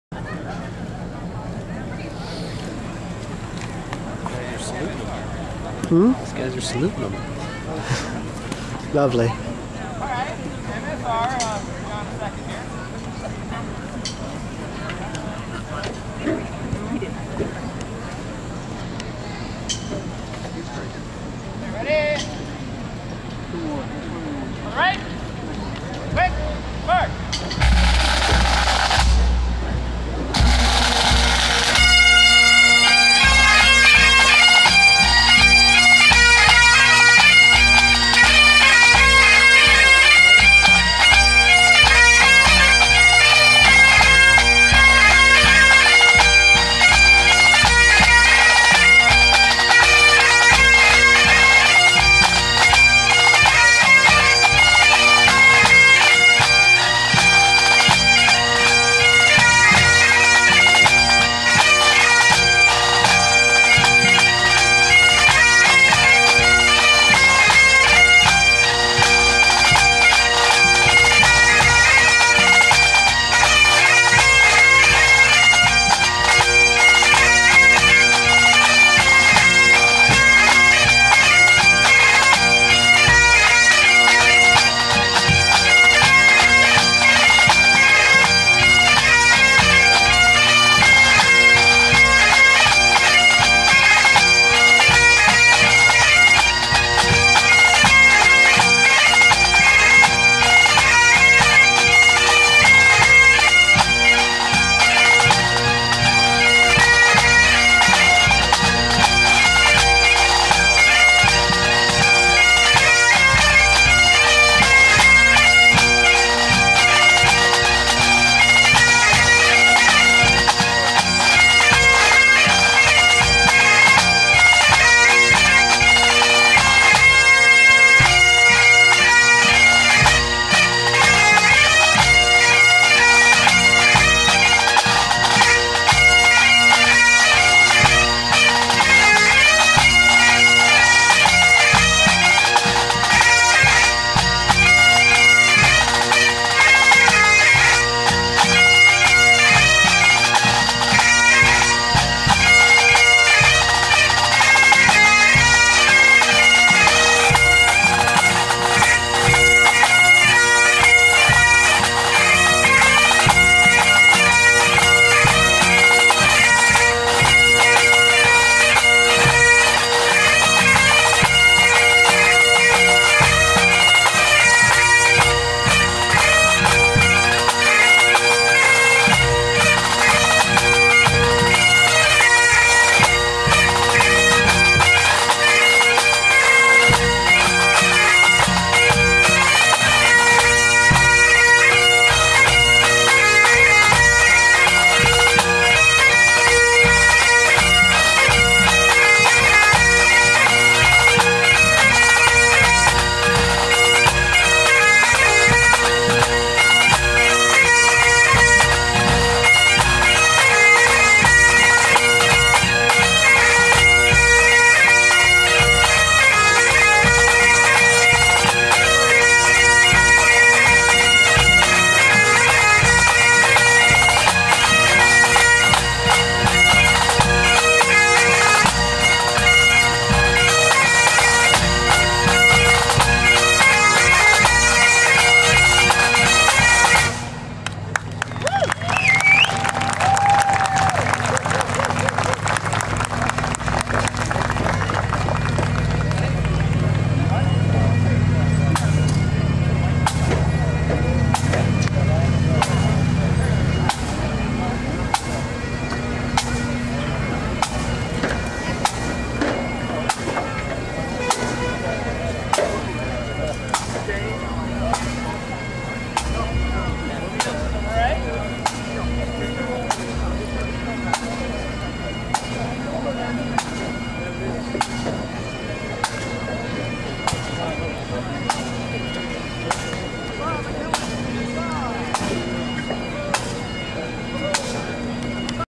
MacMillan Pipe Band - Ligonier 2007 - MSR (2nd Place)
The MacMillan Pipe Band playing in the Grade 3 MSR contest at the Ligonier Highland Games. Pipe Major Chris Hamilton, Drum Sergeant Ron Limparis. September 8, 2007 - Ligonier, Pennsylvania, USA. The band got 2nd place (behind the Balmoral Highlanders) out of six bands in the event.